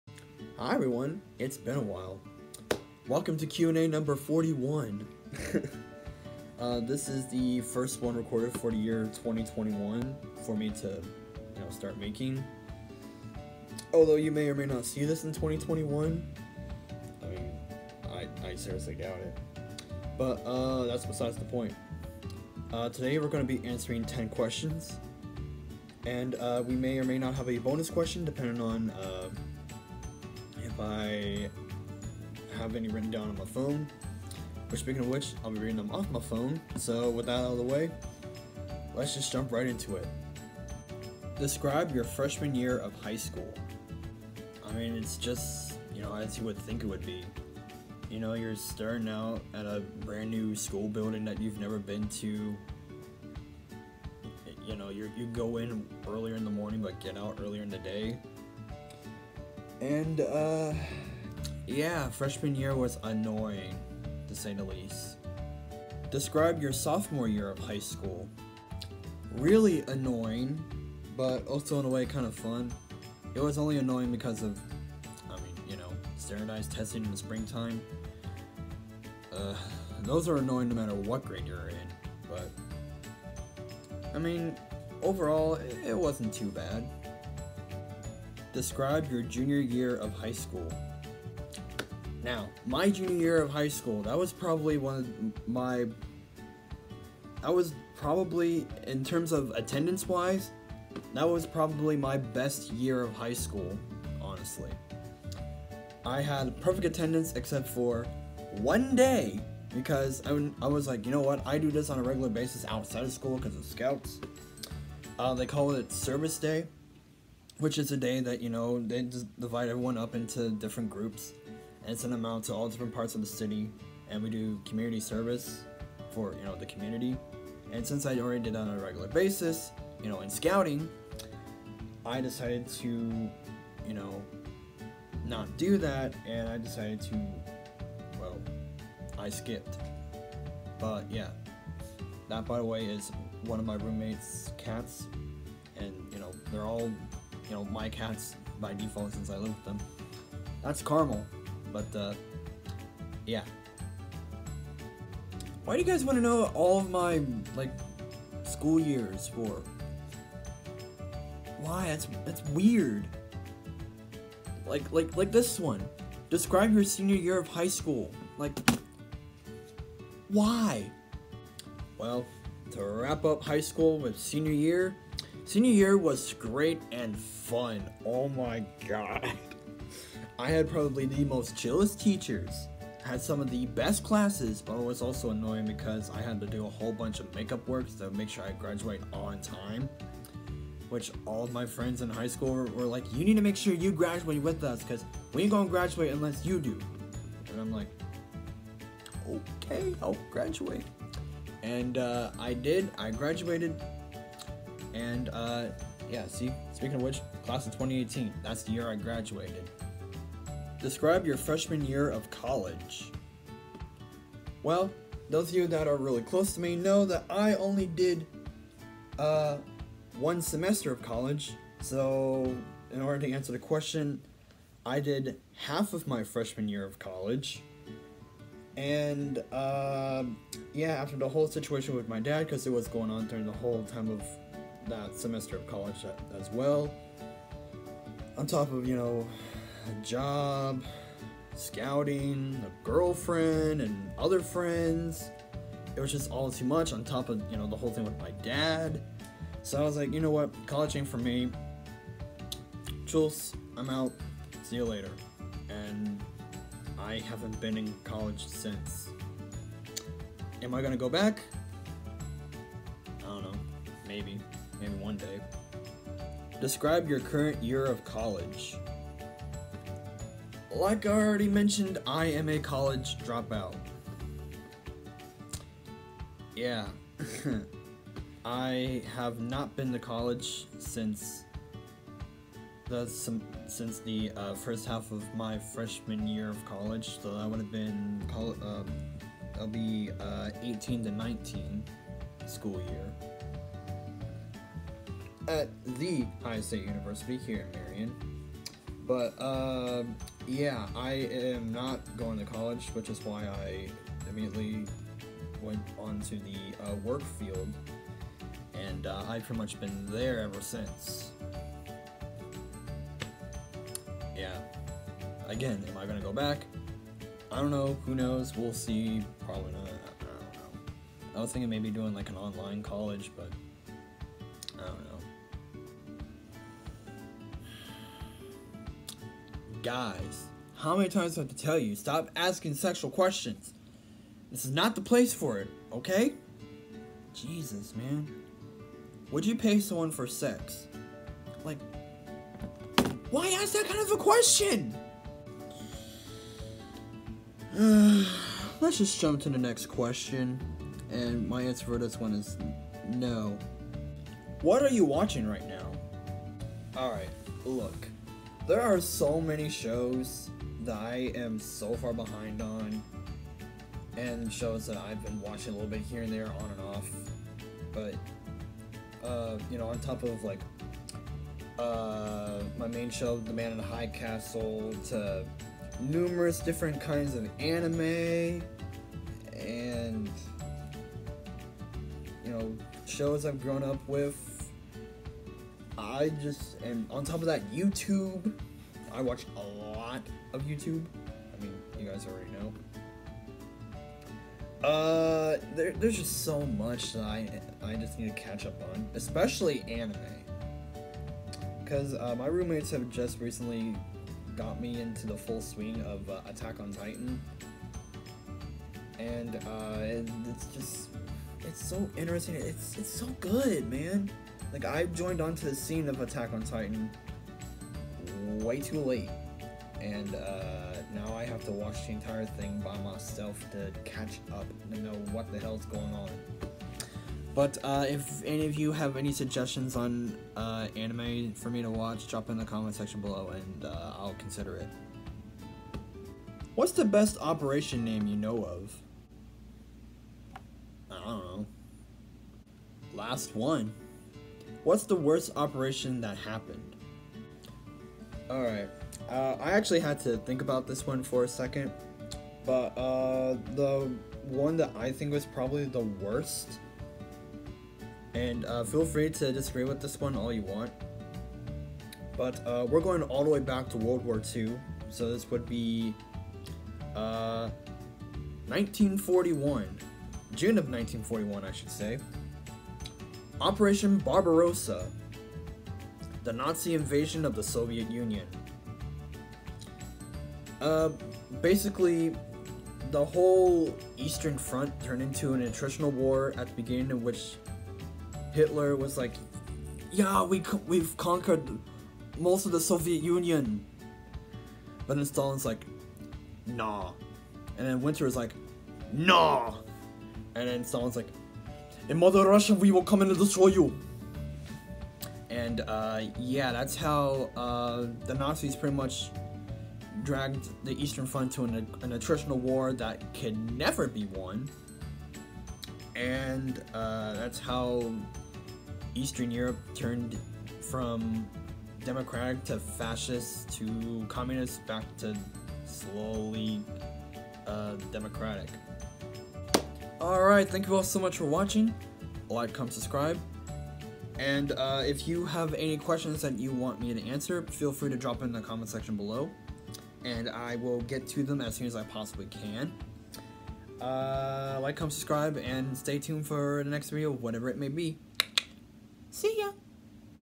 My High School & College Years (Q&A #41)
Here I answer 10 questions for you guys
If you would like to ask me a question please do so in the comments below & I will try to get to them as soon as I can